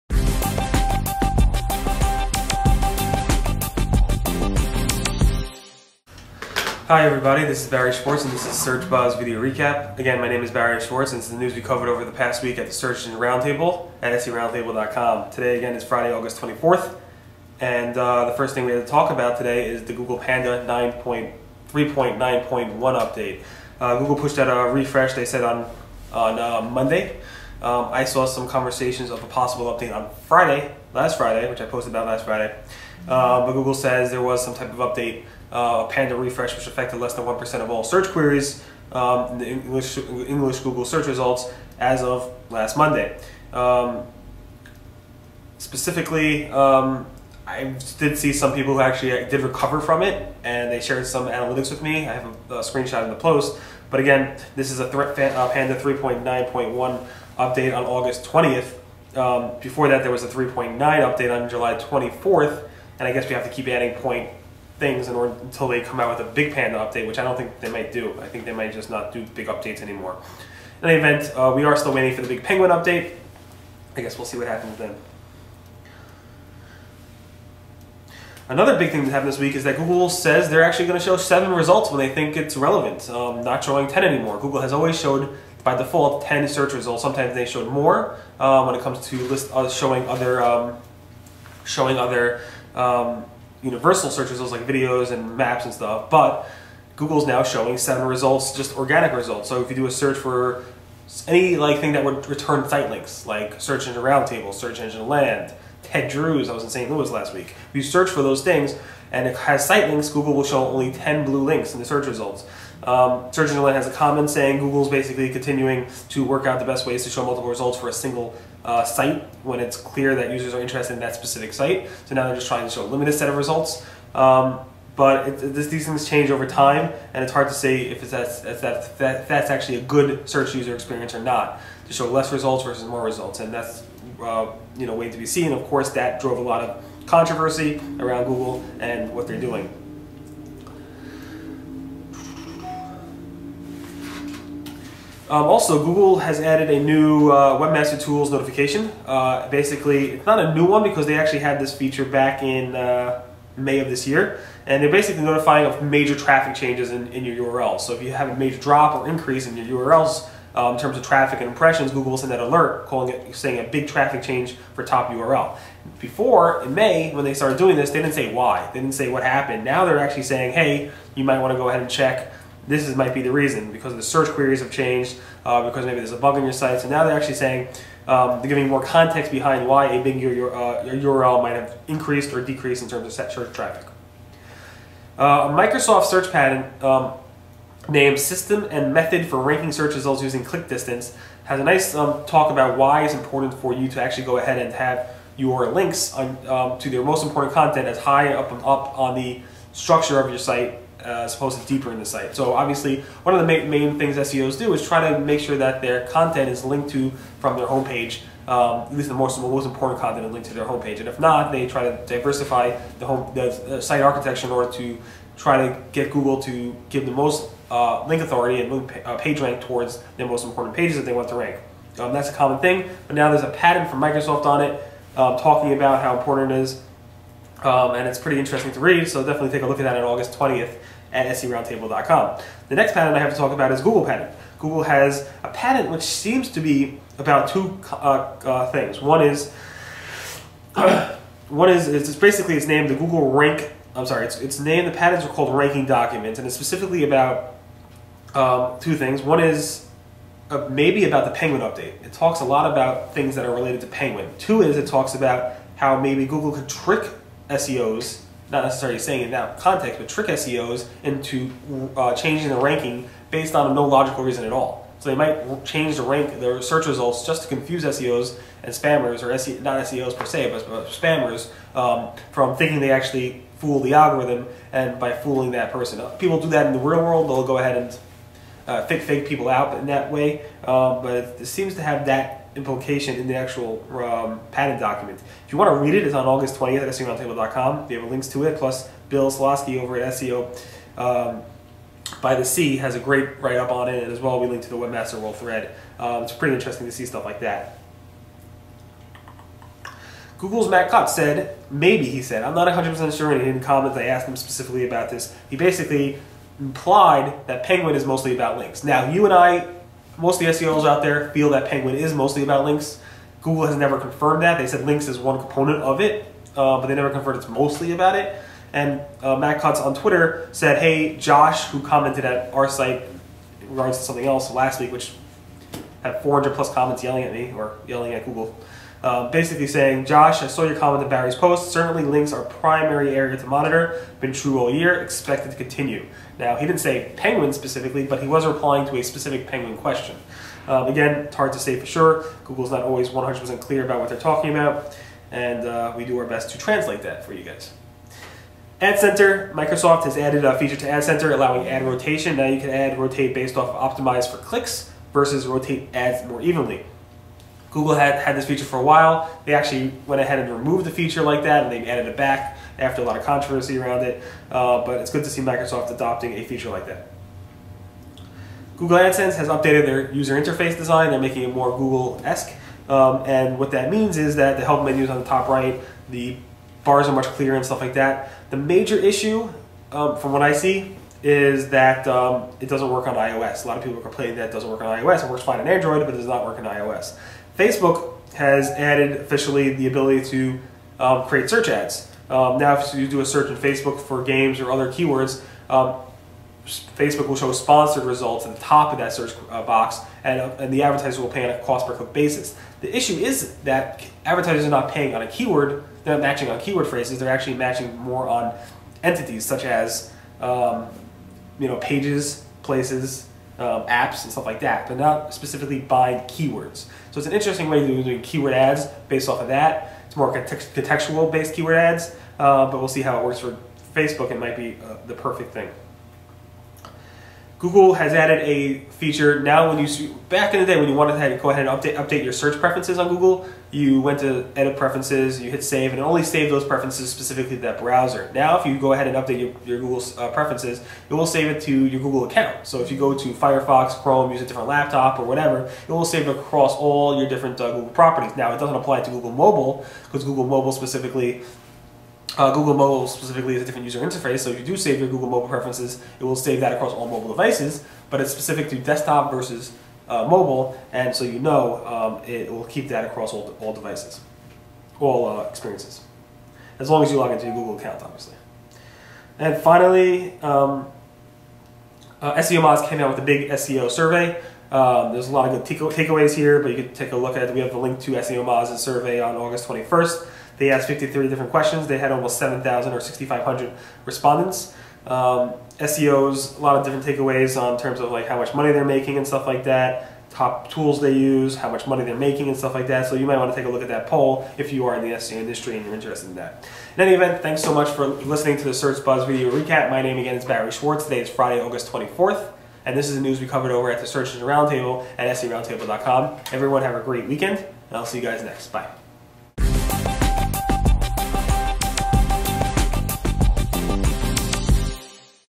Google Panda Update, 7 Google Results, Bing & Google Webmaster Tools Update & Facebook Search Ads
- This week at the Search Engine Roundtable we talk a lot about Google, some about Bing and a little about Facebook.  Google confirmed they pushed out a Panda update this past Monday.  Google is showing seven results for searches that return sitelinks.  Google Webmaster Tools improved search query notifications.  Bing Webmaster Tools added percent changes to the dashboard reports.  A Microsoft search patent talks about click distance and a Google search patent talks about Penguin and tricking SEOs.  Google's Cutts said Penguin is mostly about links - or maybe he didn't say that? Microsoft adCenter now has ad rotation preferences.  Google updated the AdSense user interface and it breaks on iOS.  Google saves your search preferences to your Google account now.  Finally, the big SEO survey is now out on SEOmoz.  That was this past week at the Search Engine Roundtable.

Patent: Google Faking Search Results To Trick SEOs?
Google's Cutts Say Penguin Is About Links?